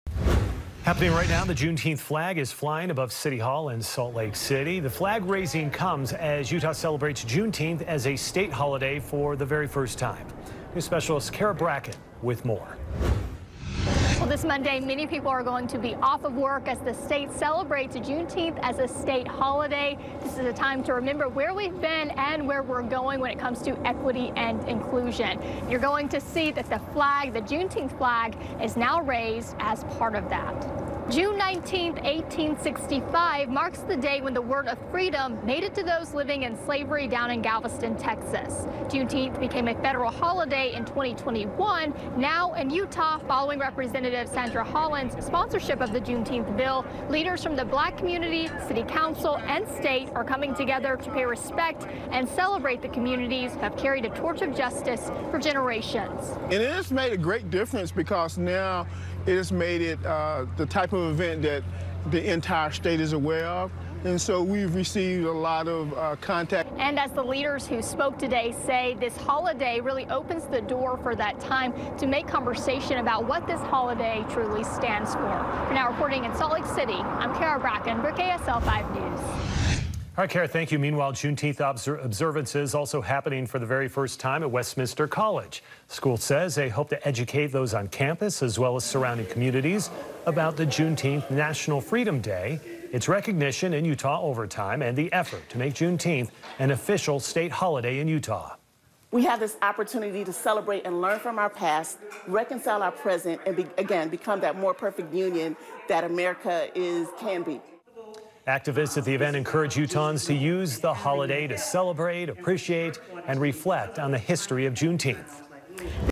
Utah celebrates Juneteenth for the first time as an official state holiday
State leaders raised the Juneteenth Flag above City Hall in Salt Lake City Tuesday.
The flag-raising comes as Utah celebrates Juneteenth as a state holiday for the first time.